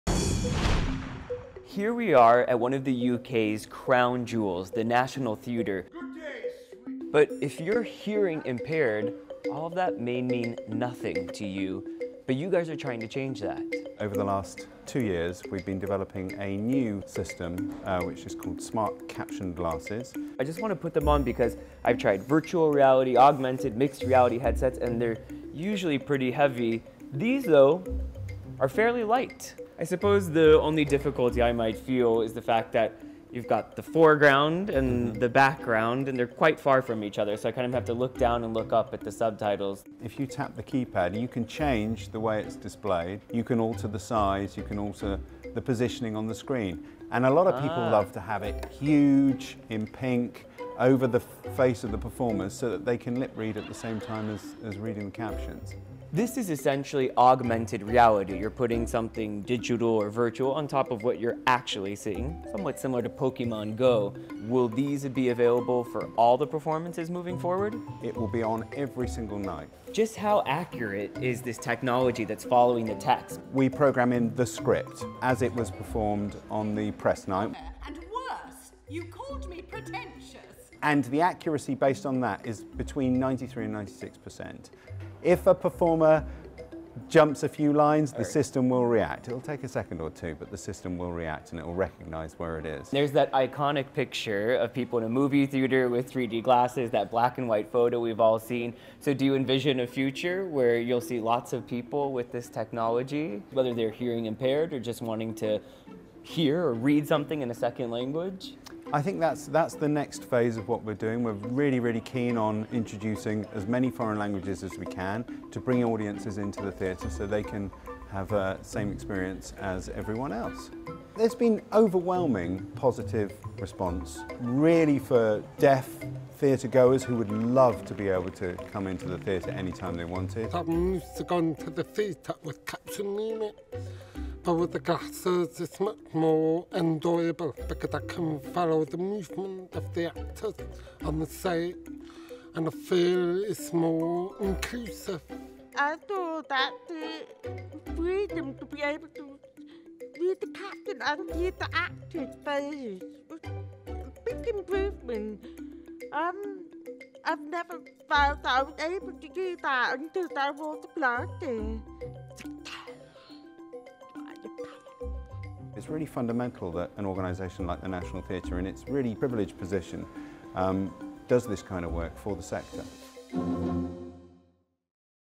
Explaining Glasses That Display Captions
These smart glasses display captions so an audience member can read them and watch a performance at the same time. Samuel Burke goes to the National Theatre to try them out.